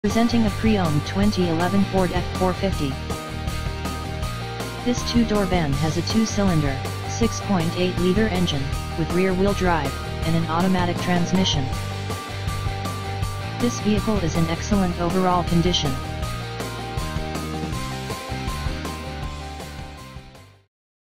2011 Ford F-450 Handicap lift bus for sale in CLINTON TOWNSHIP, MI
Great running Bus, with Handicap lift, Side door, rear exit, has Fiber Glass turtle top conversion 23 seat pas. The lift is worth 10,000 by its self and you can own the whole bus for 13,995!